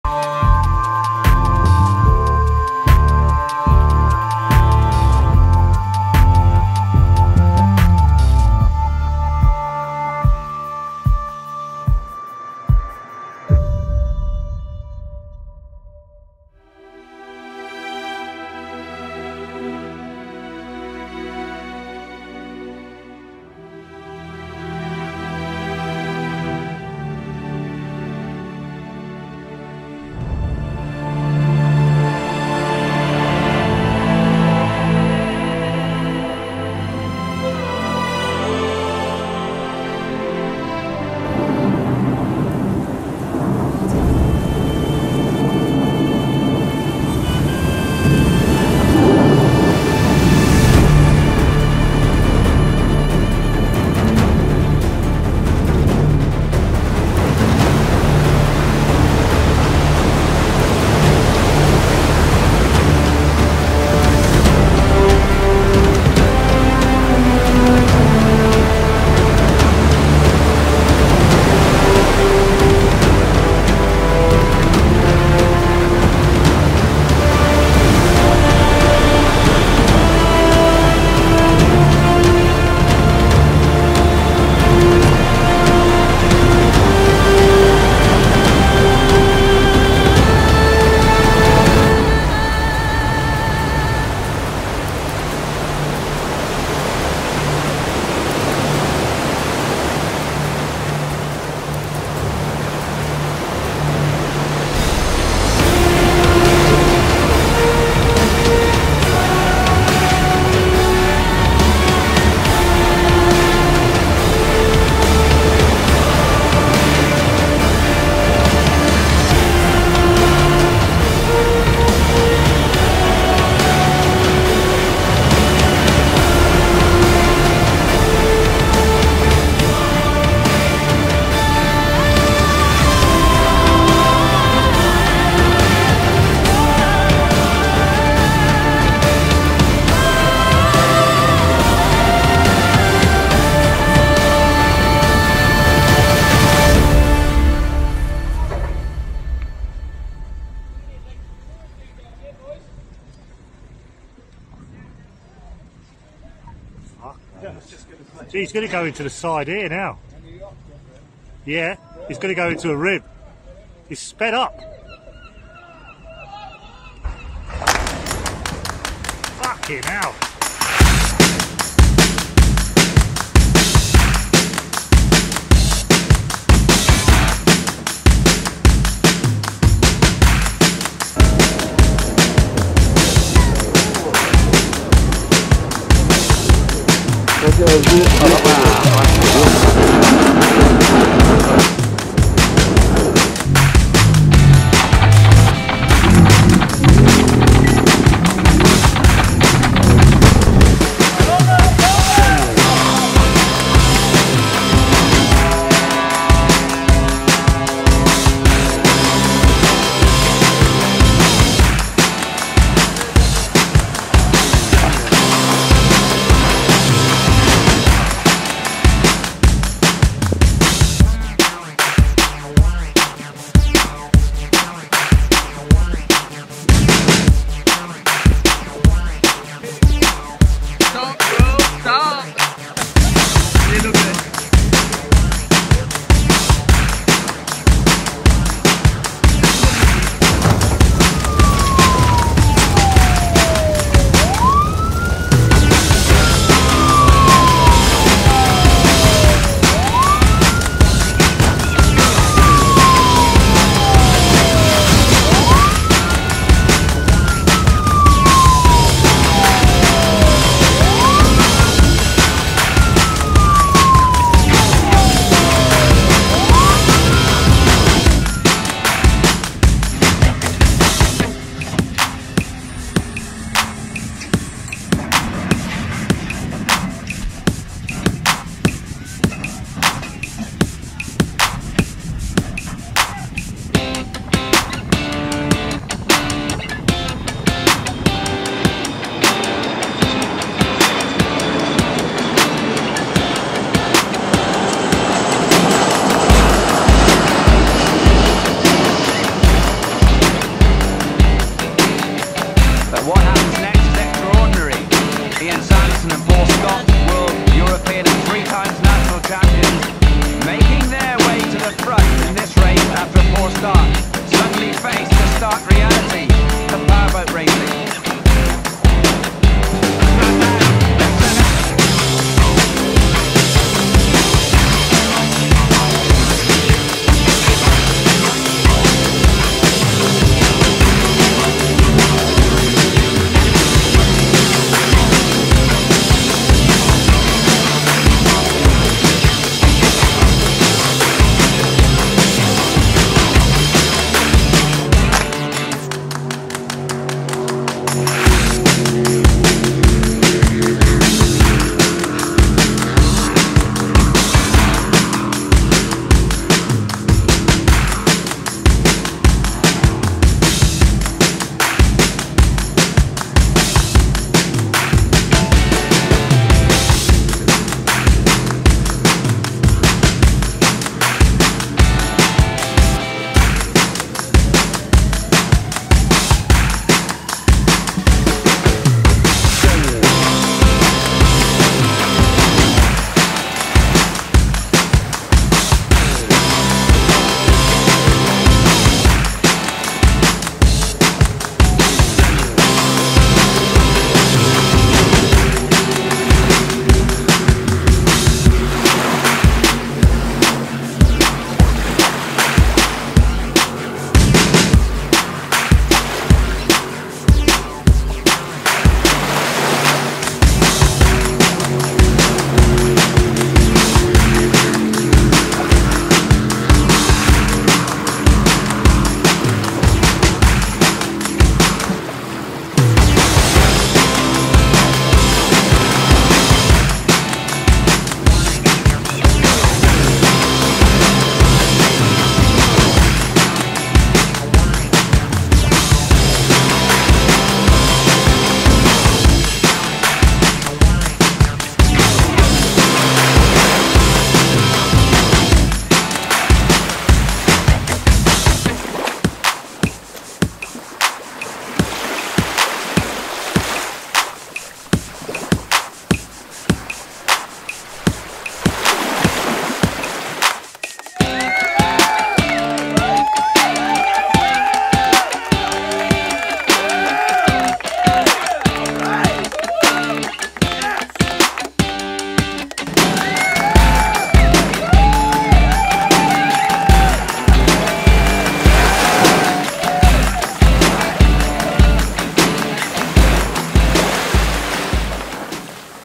Idiots on Boats | Compilation
Compilation Boat Crash
I hope you will like this video. Any feedback is appreciated. Have fun all !!! These people cause serious damage to their boats and other boats while out on the water. Make sure to watch until the end as you won't believe why this boat sinks...